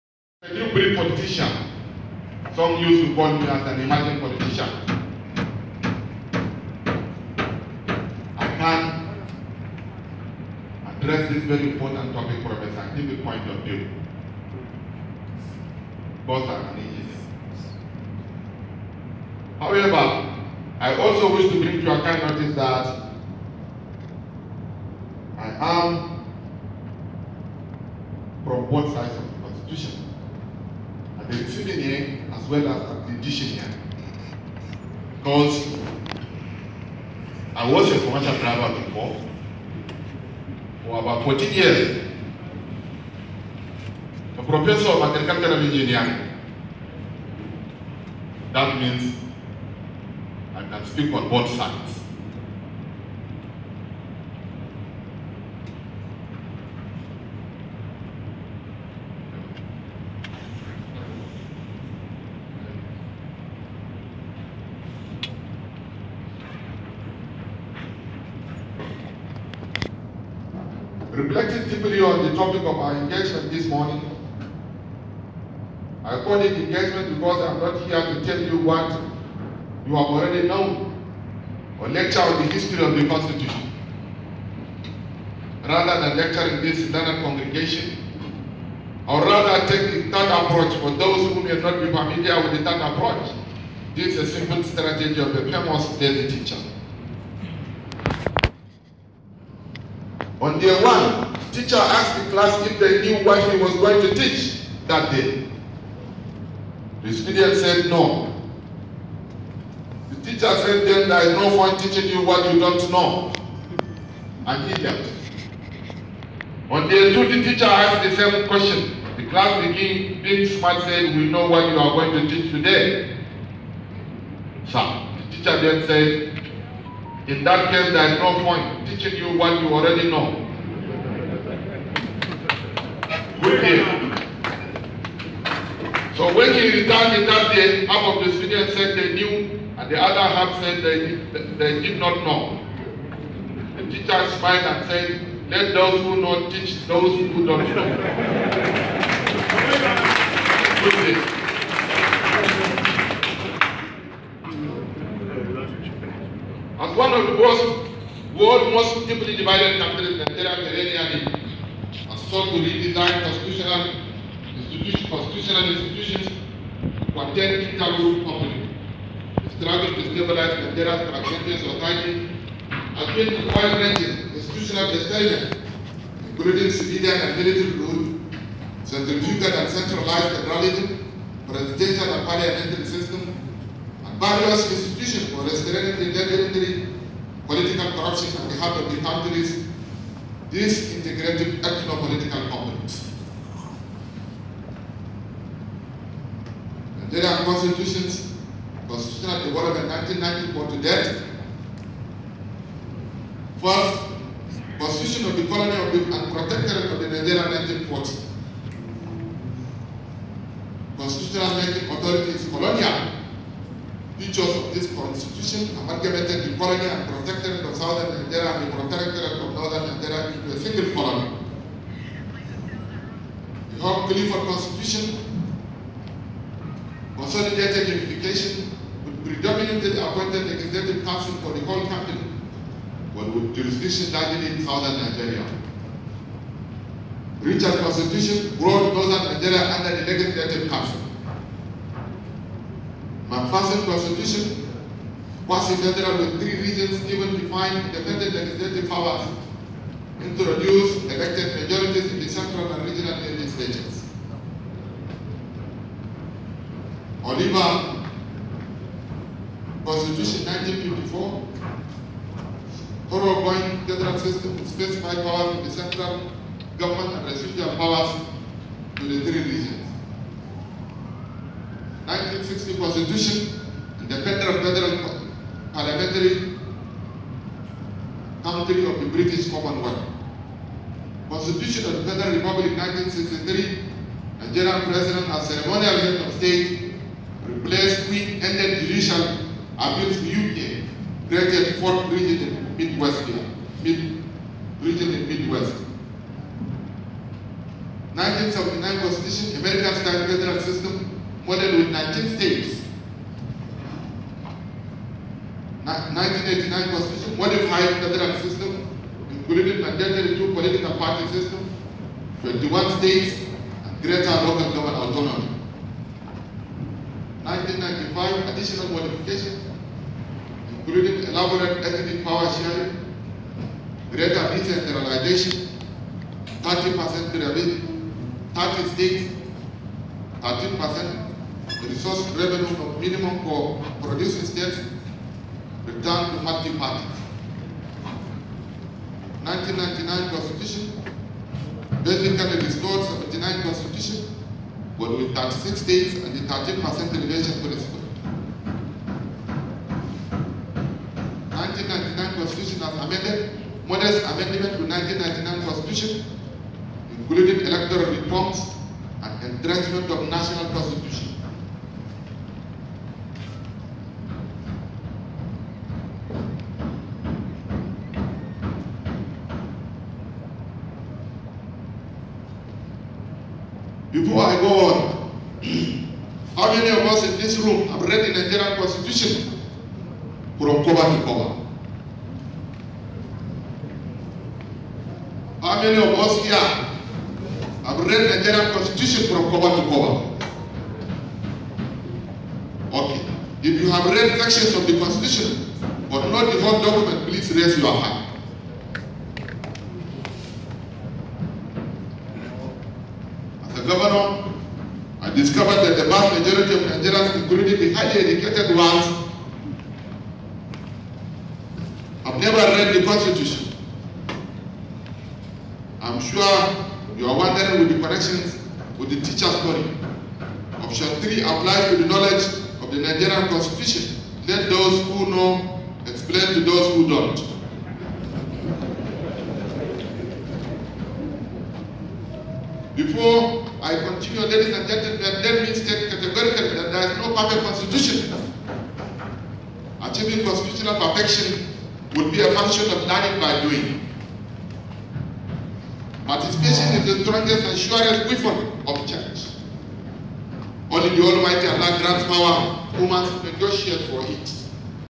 17th Gani Fawehinmi Annual lecture
The lecture was based on the constitutional history of Nigeria Dysfunction: Any Pathway to Indivisibility and common progress.